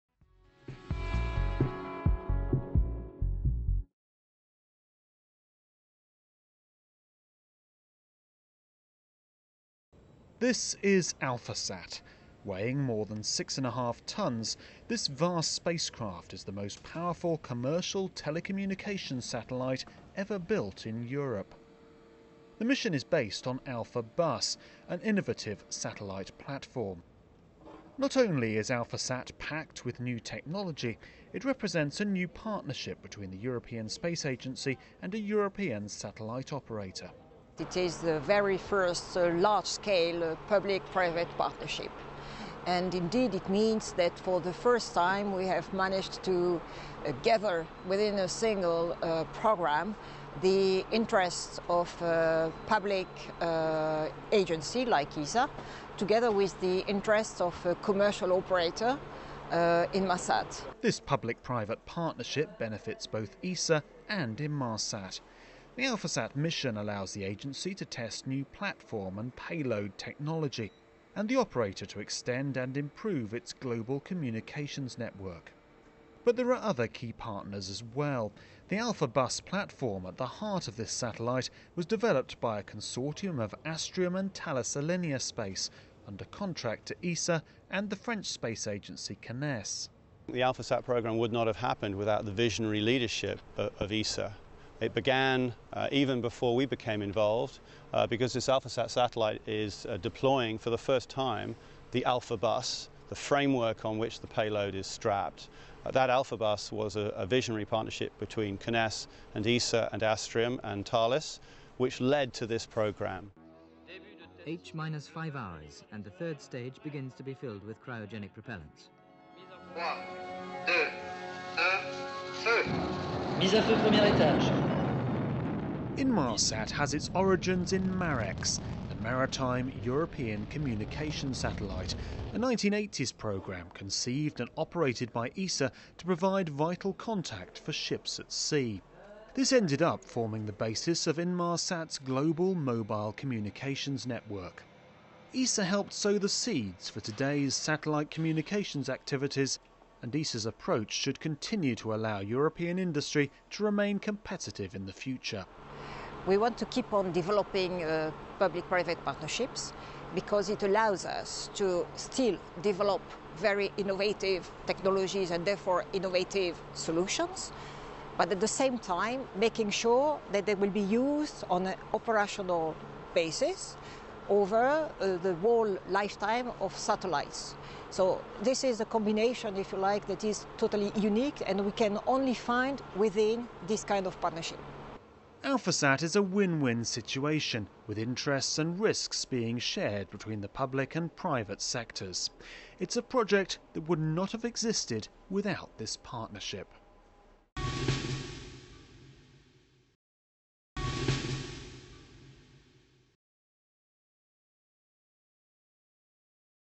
Alphasat: new partnership in SatCom
Alphasat, the most sophisticated commercial communications satellite ever built, is due for launch at the end of July. The mission is a partnership between the European Space Agency and satellite operator, Inmarsat, and will be the first flight of Europe's powerful new Alphabus satellite platform. The public-private partnership that made Alphasat possible will put the European space industry in a strong position for the future.